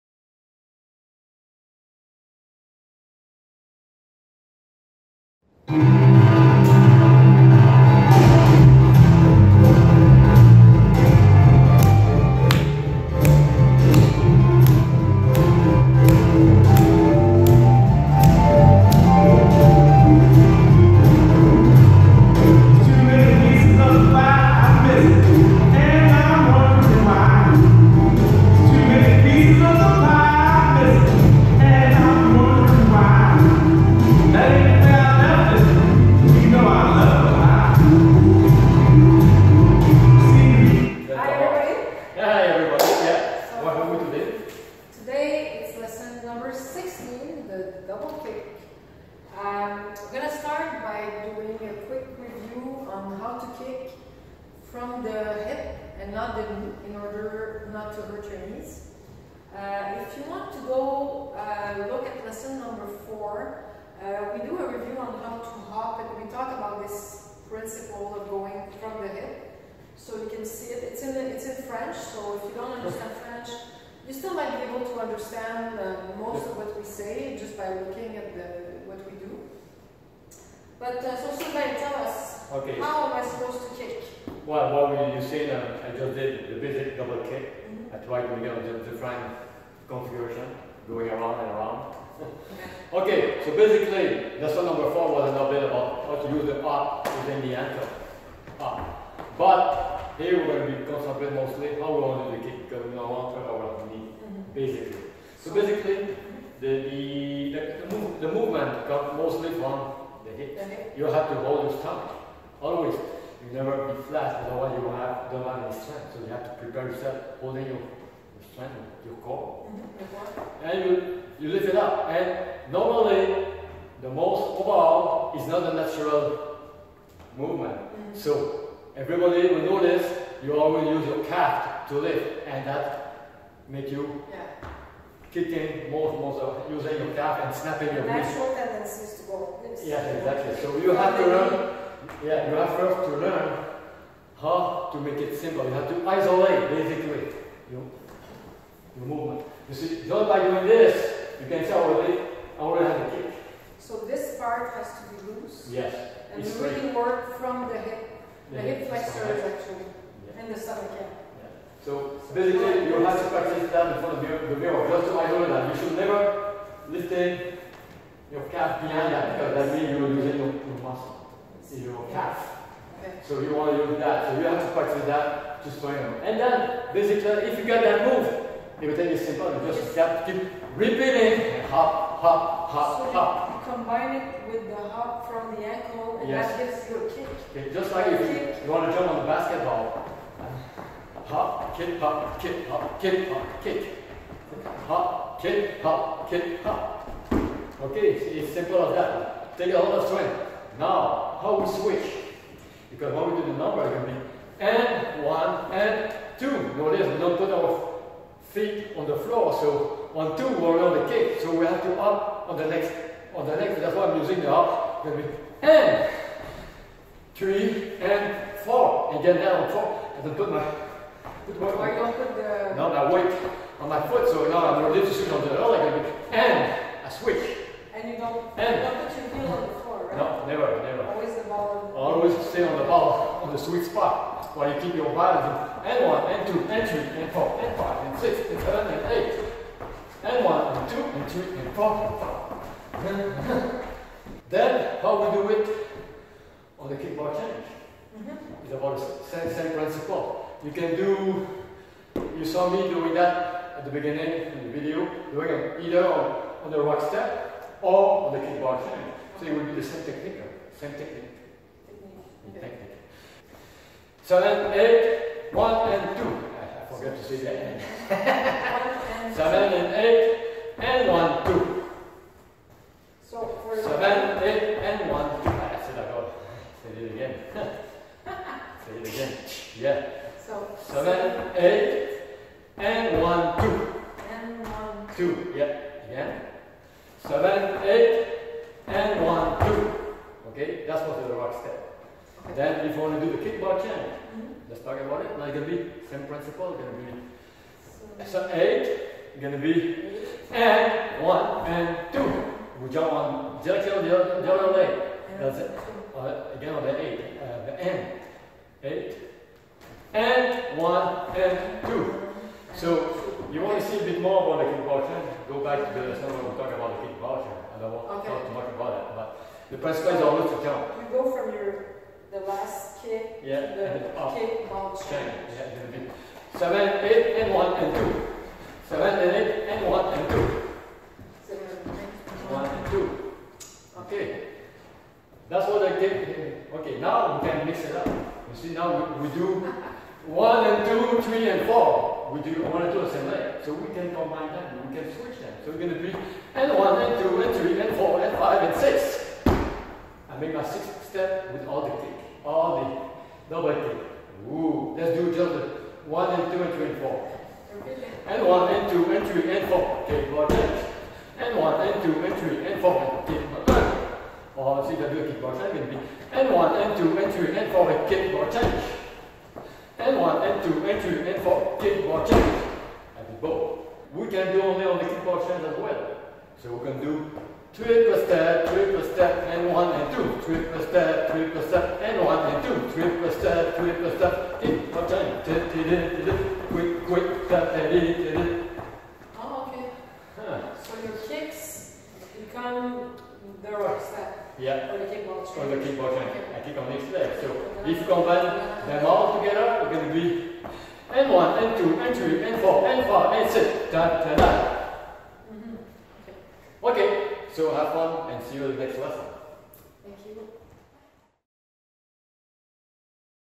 Boogie Lesson #16 - Double Kick
This video is for Advance dancers who master the previous lessons #4 and #5.It is a simple step, however it request a good foundation on the bouncing technic to make it looking smooth and slick.